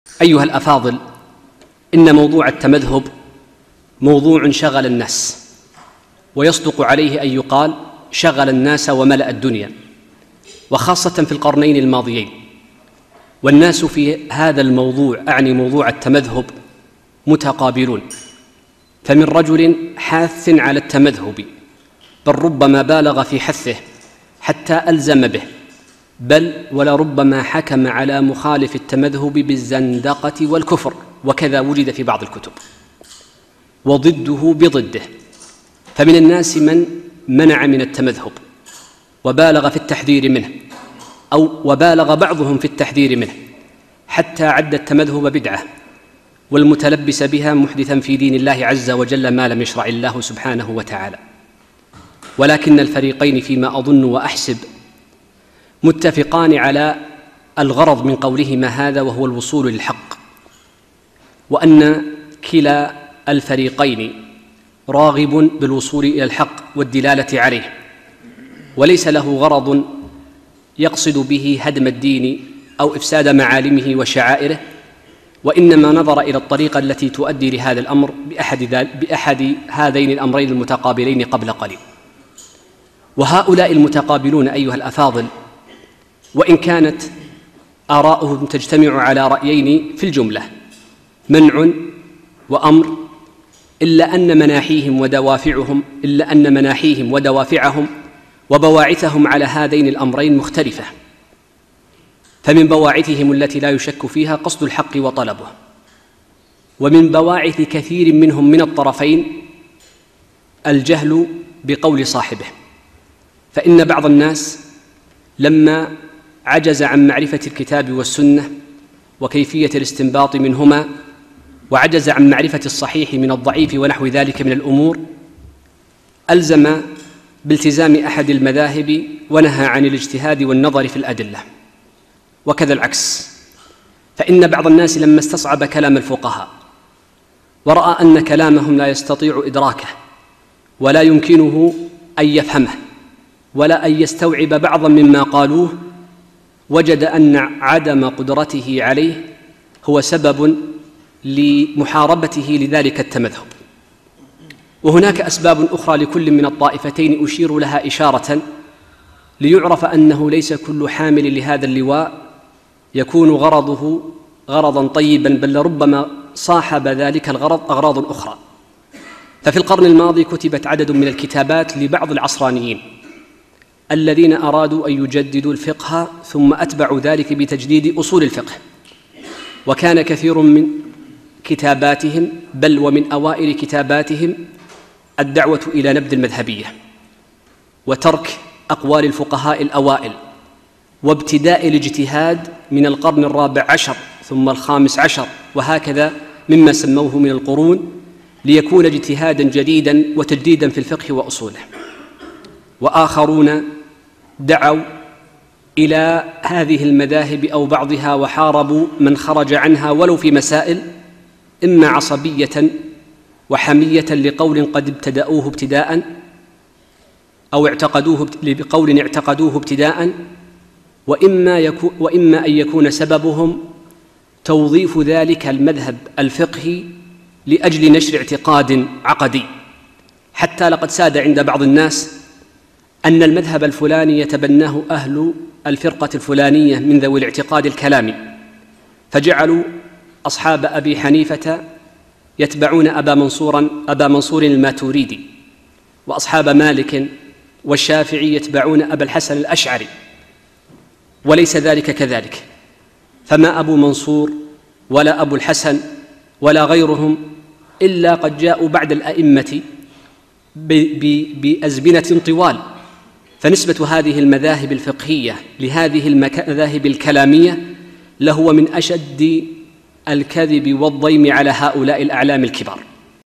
الشيخ د. عبد السلام الشويعر - النزاع في موضوع التمذهب
‼️النزاع في موضوع التمذهب 📚

الشيخ أ. د.